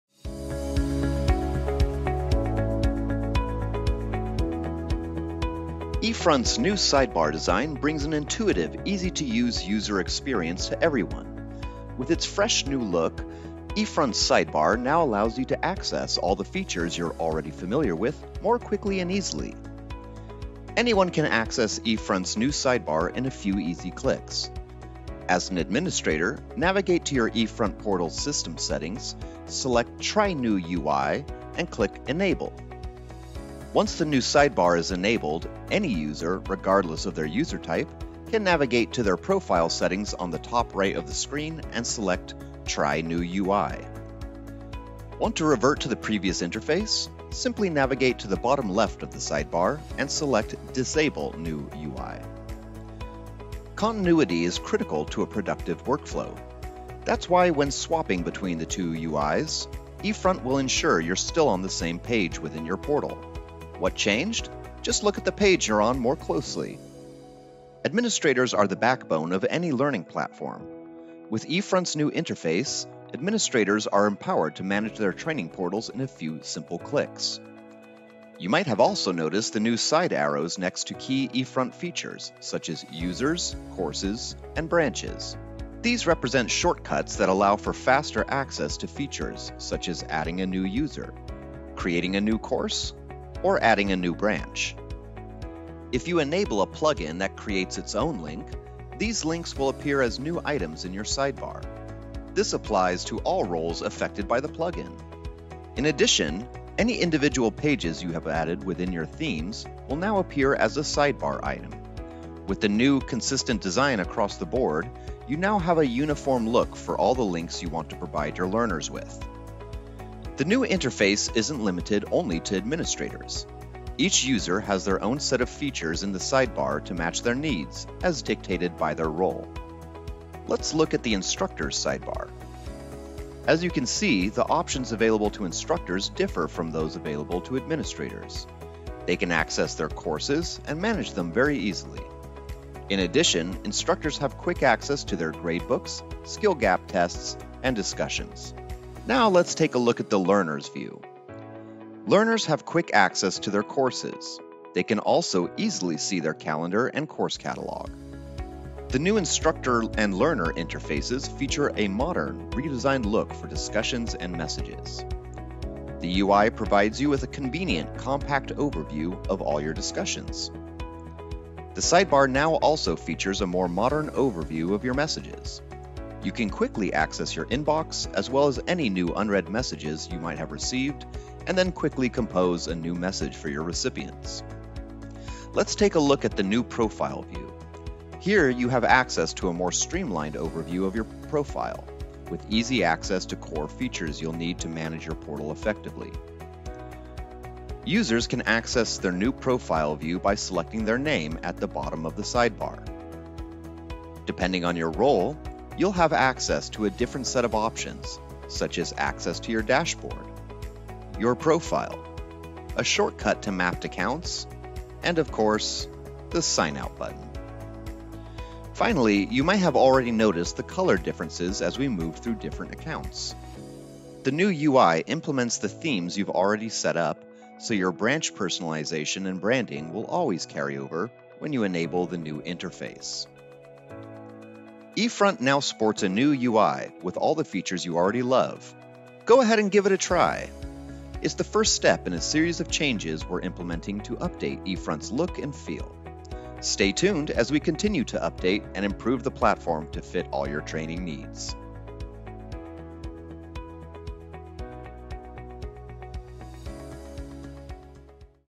Discovering the new eFront sidebar
eFront interface is changing! Take a look at the new intuitive and easy-to-use sidebar.
Create your online course in no time and start your training today!